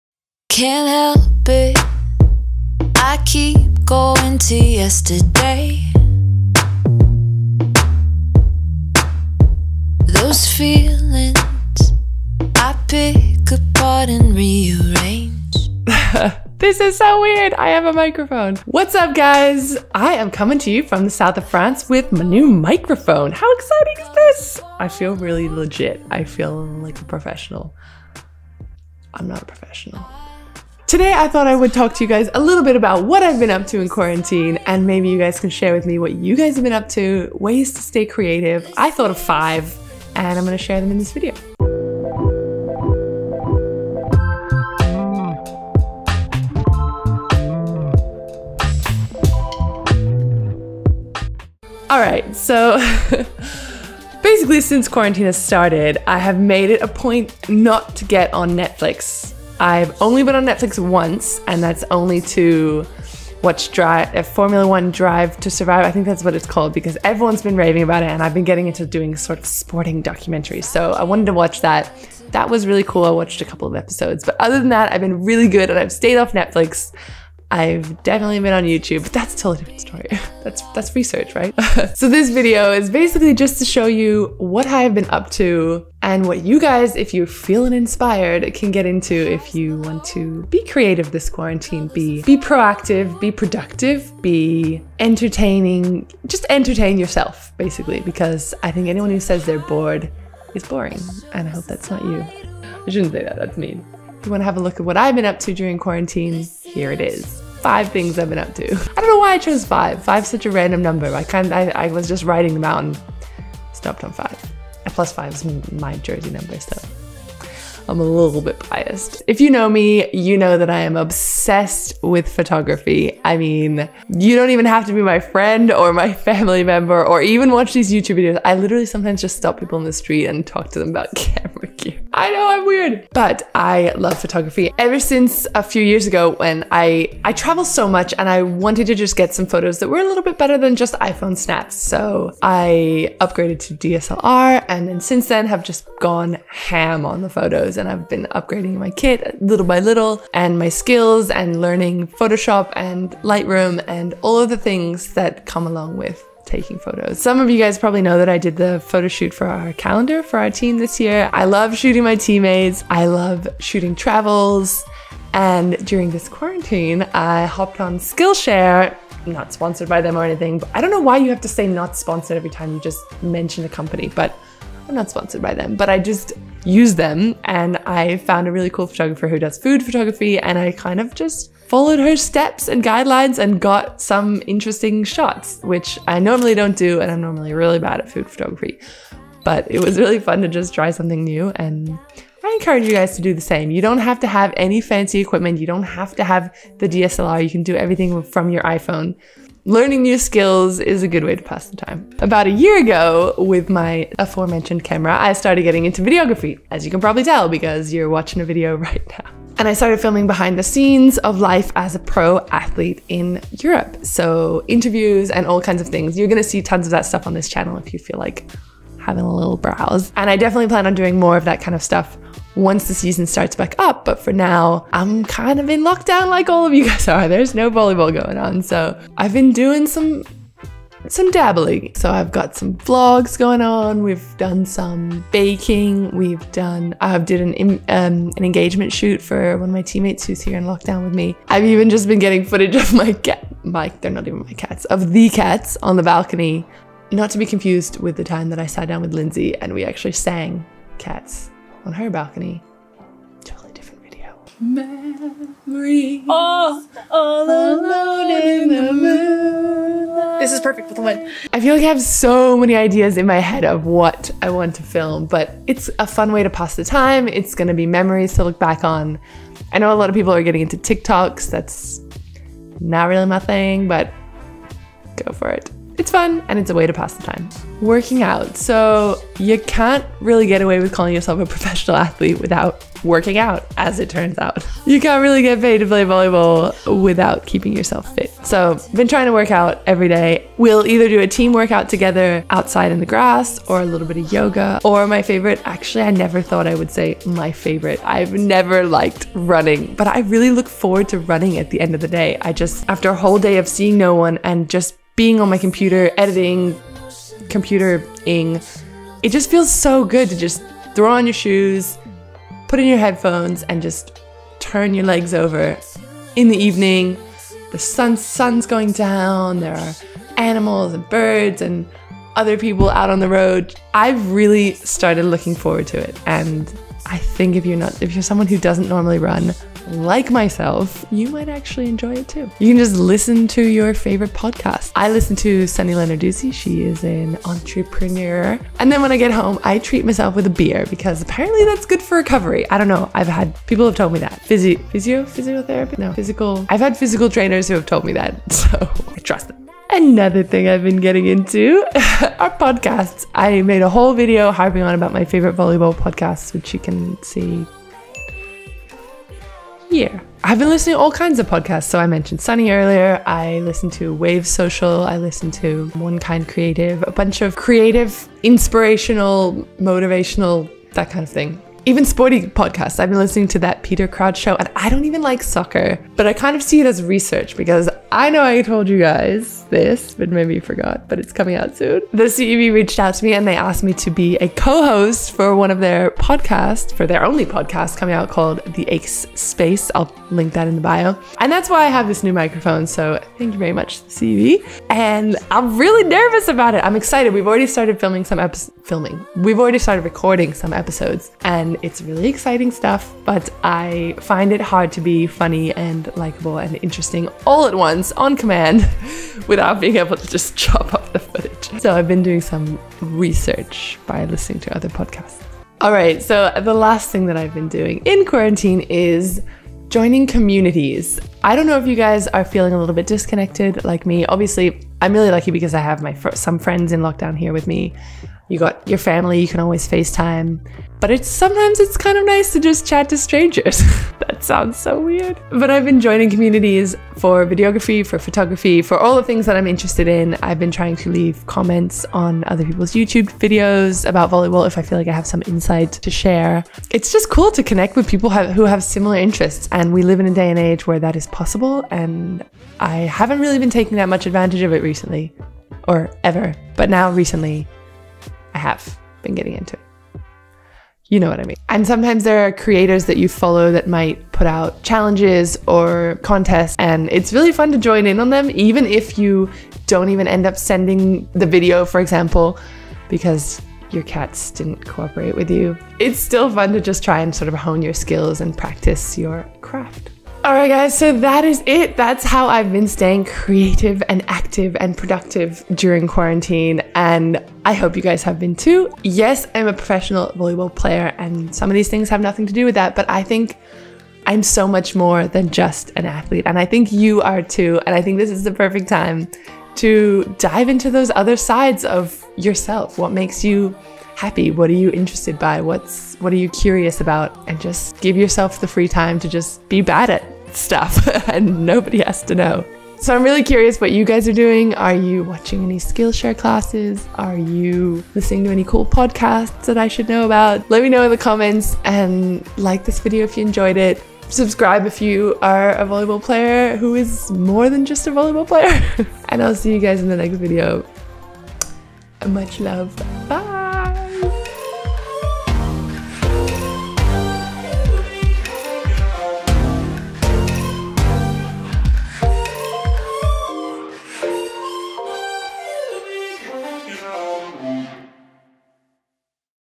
5 Ways to Stay Creative During Quarantine from a PRO ATHLETE.
Just as the title suggests! Here are 5 ways you can stay creative, active and productive during quarantine! Am I a professional volleyball player? Yes. Do all of these have to do with volleyball? No. But creativity is fun.

Thanks for stopping by to watch. Wilson, I'm looking at you. Did I write volleyball enough times in this description? Apparently the algorithm likes that. Volleyball. K bye.